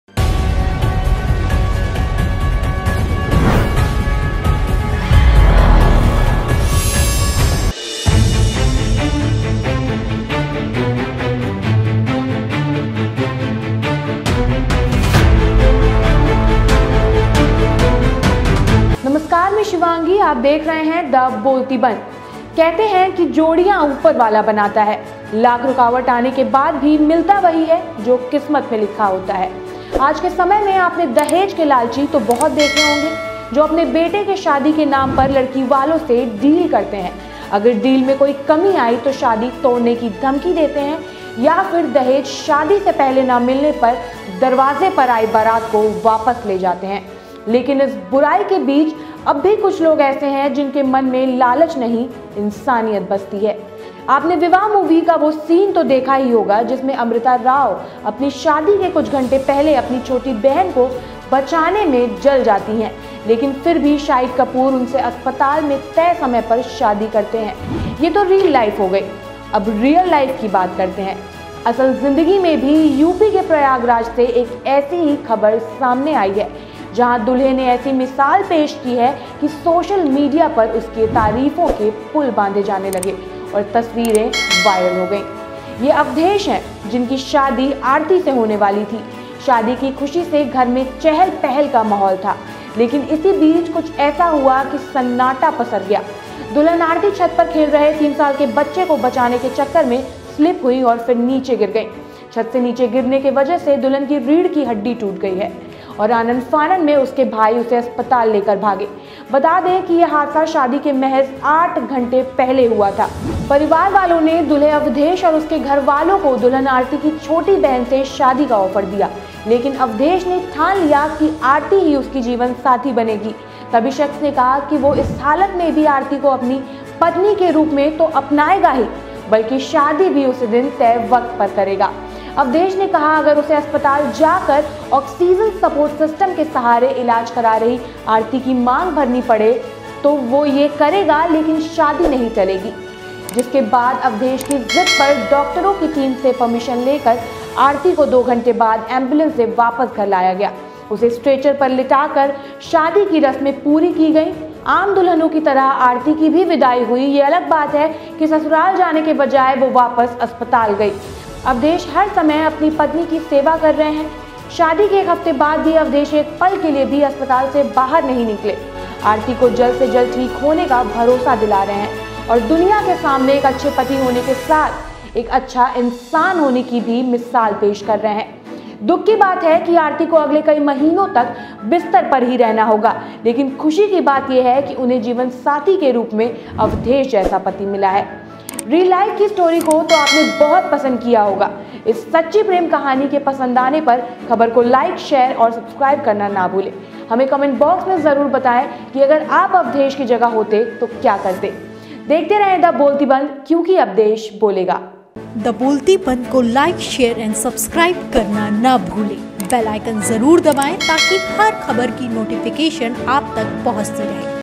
Prayagraj's man marries bride on a stretcher | brides backbone broken hours before the wedding
Prayagraj Man Marries Bride on Stretcher । Bride Breaks Spinal Cord Hours Before Wedding । Viral Love Story